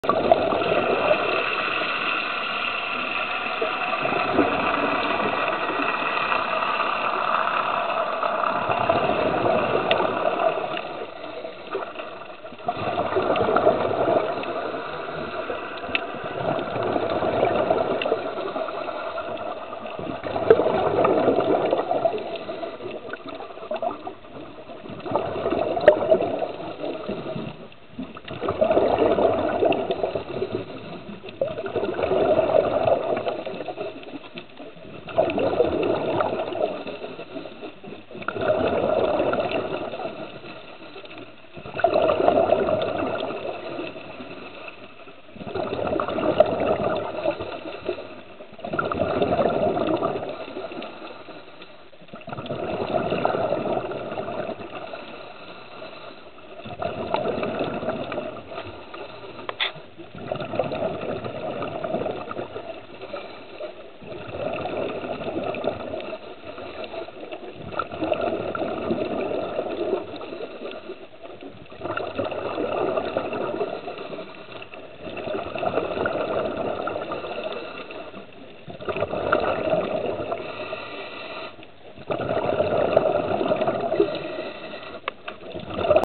Mari & Luc at Christ of the Abyss
At John Pennekamp State Coral Park in Key Largo. About 25' deep, accessible by snorkeling too.